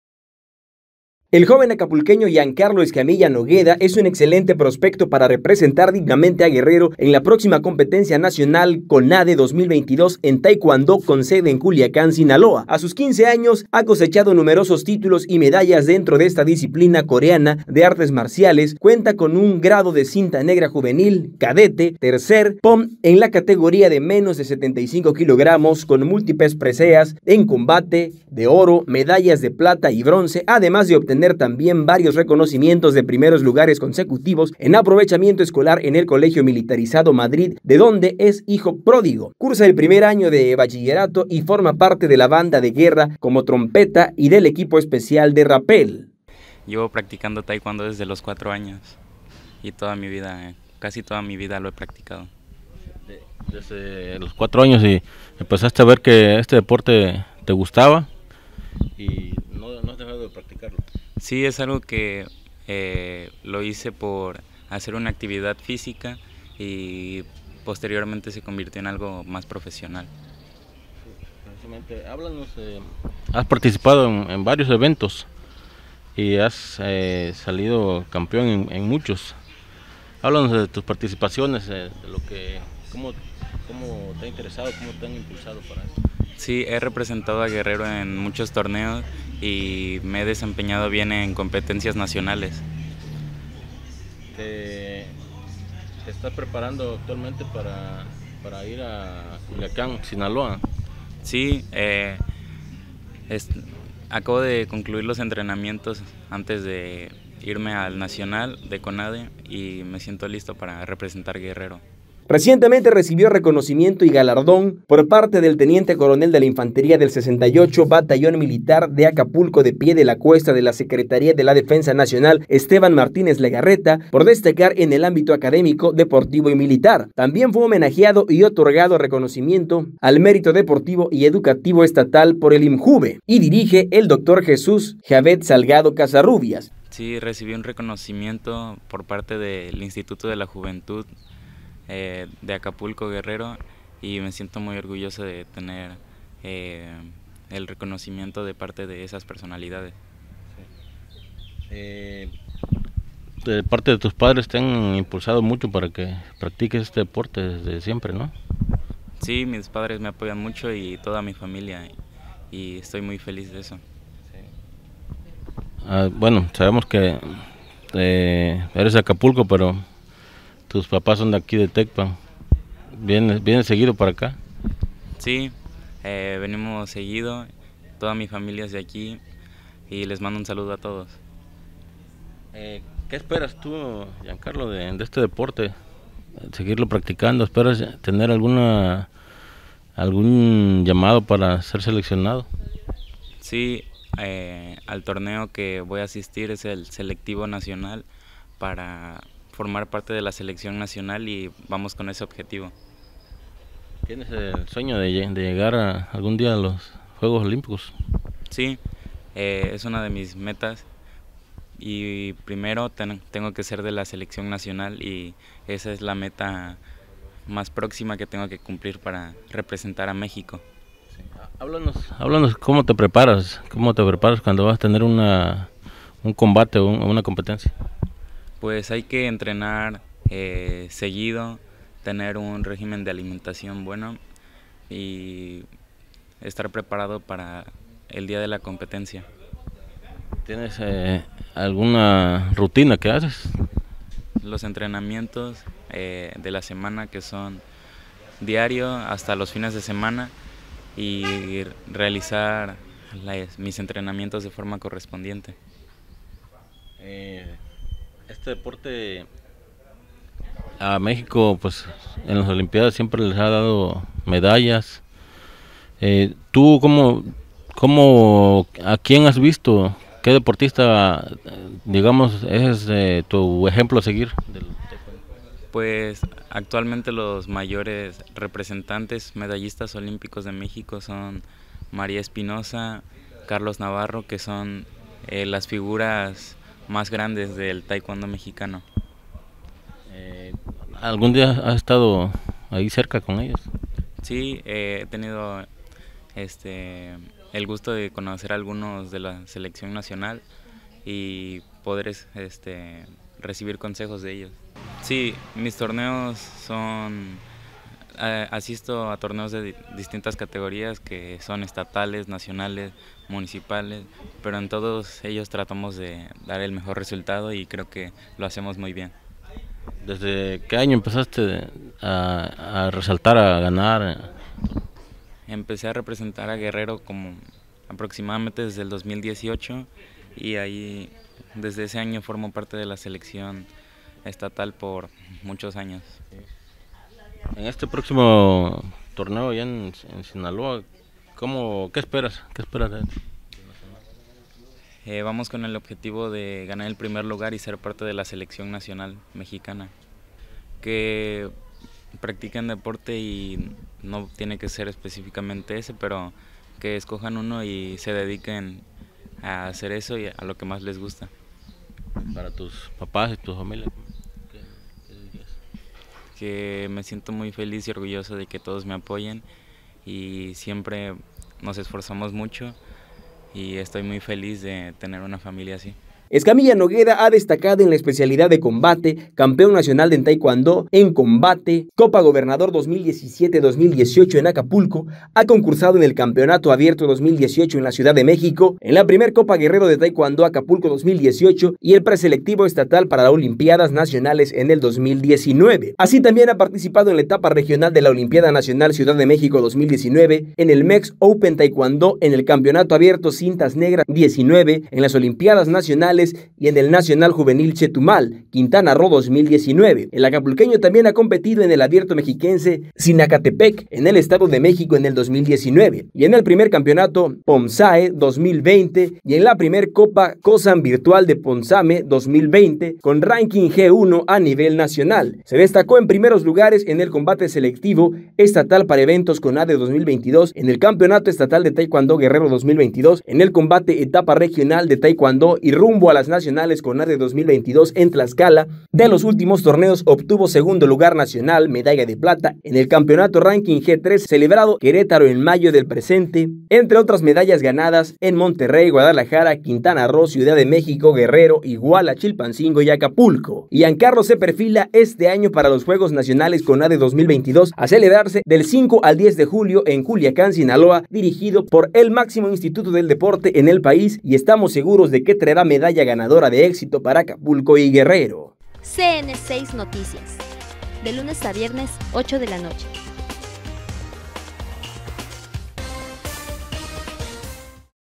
Giancarlo Escamilla Nogueda es un excelente prospecto para representar dignamente a Guerrero.
Giancarlo Escamilla Nogueda es un excelente prospecto para representar dignamente a Guerrero en la próxima competencia Nacional CONADE 2022